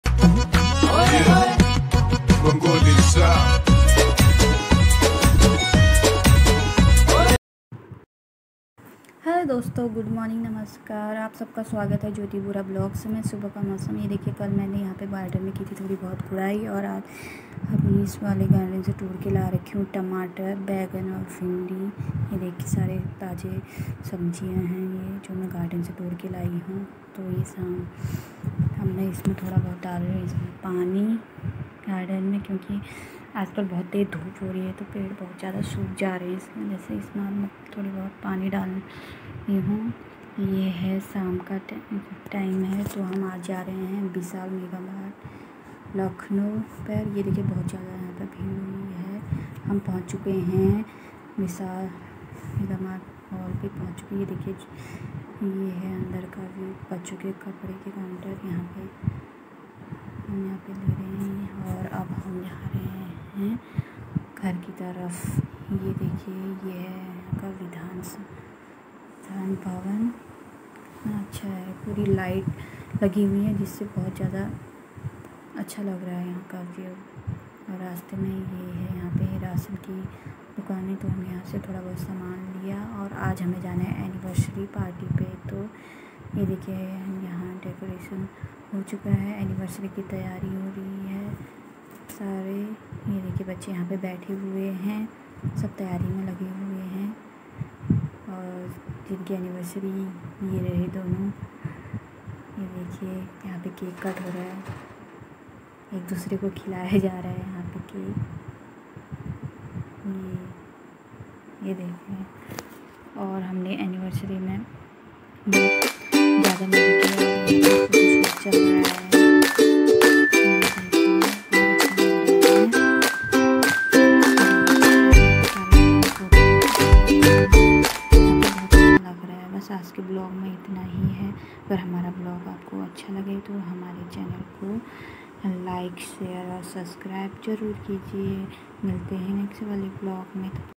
आज हम लोग गए शादी की सालगिरह समारोह में🥰
आज हम लोग गए शादी की सालगिरह समारोह में🥰🥳🥳🎉🎉🎉🎉।। Jyoti Bora Vlogs ।।